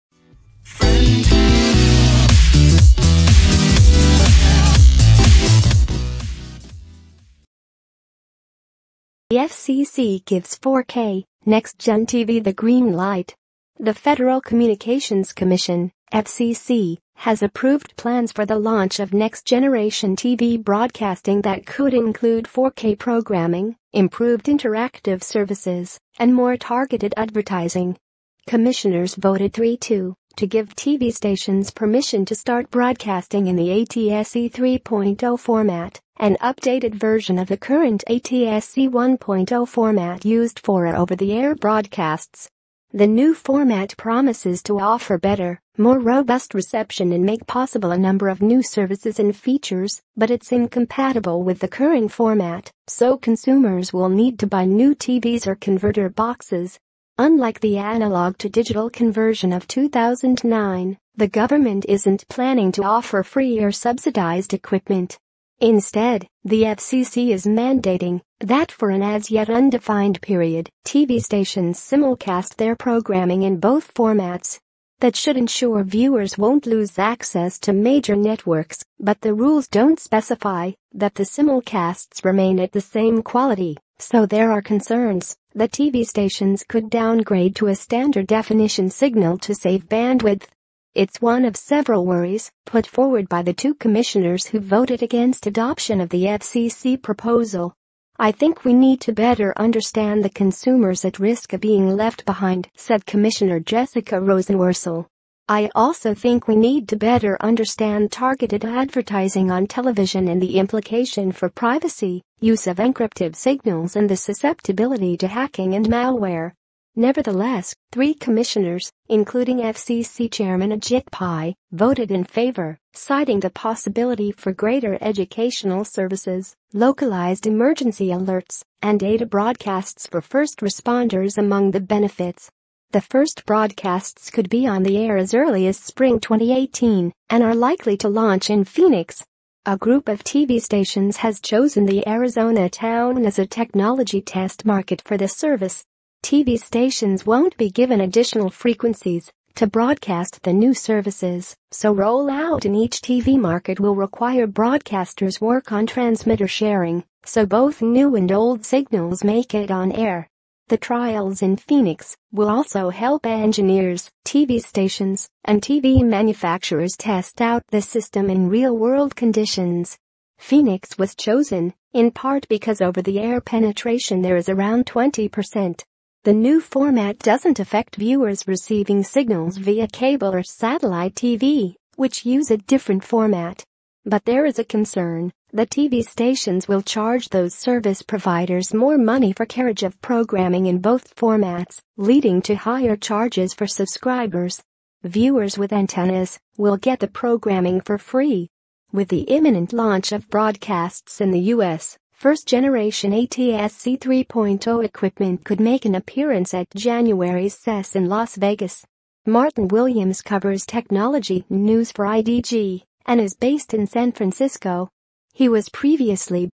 The FCC gives 4K, next gen TV the green light
The Federal Communications Commission (FCC) has approved plans for the launch of next-generation TV broadcasting that could include 4K programming, improved interactive services, and more targeted advertising.

Commissioners voted 3-2 to give TV stations permission to start broadcasting in the ATSC 3.0 format, an updated version of the current ATSC 1.0 format used for over-the-air broadcasts.

The new format promises to offer better, more robust reception and make possible a number of new services and features, but it's incompatible with the current format, so consumers will need to buy new TVs or converter boxes. Unlike the analog-to-digital conversion of 2009, the government isn't planning to offer free or subsidized equipment.